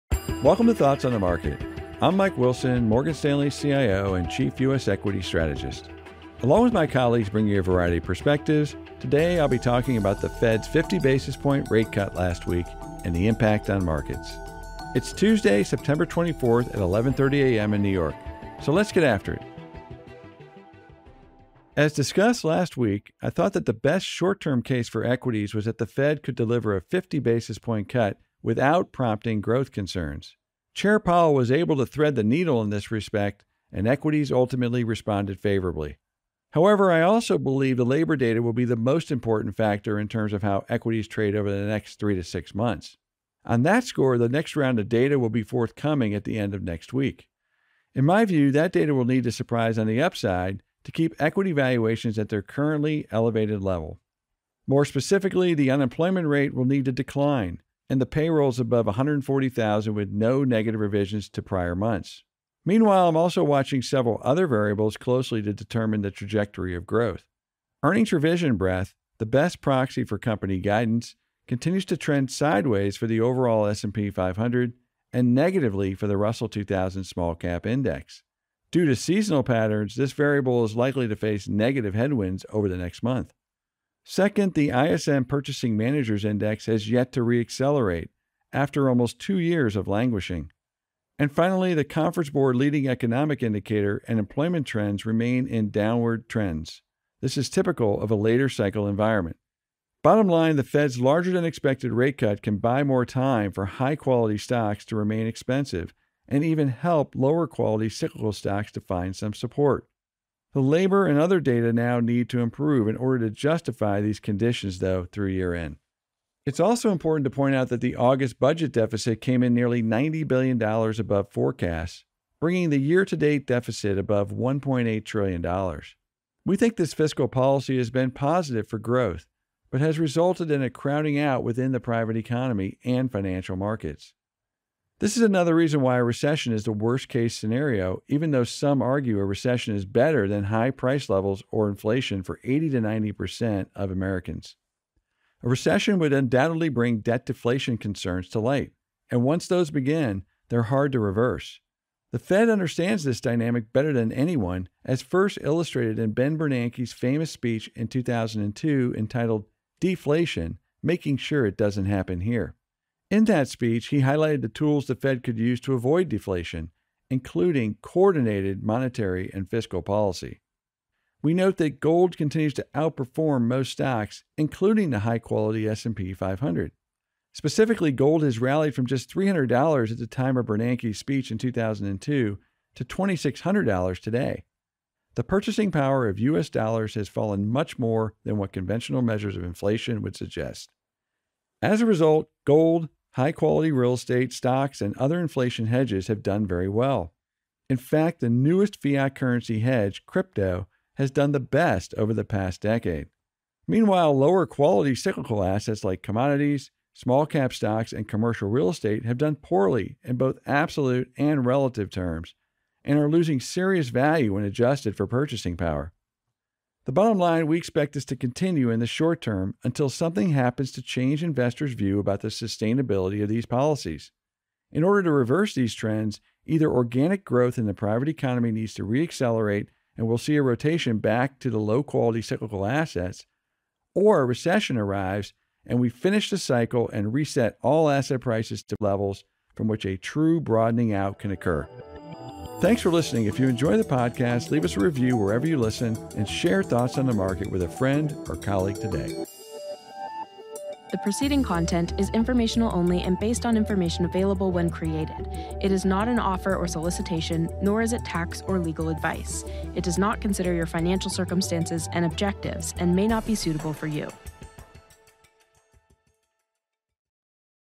One Rate Cut, Many Effects
From stock price fluctuations to concerns about deflation, the reactions to the Fed rate cut have been varied. But we still need to keep an eye on labor data, says Mike Wilson, our CIO and Chief U.S. Equity Strategist.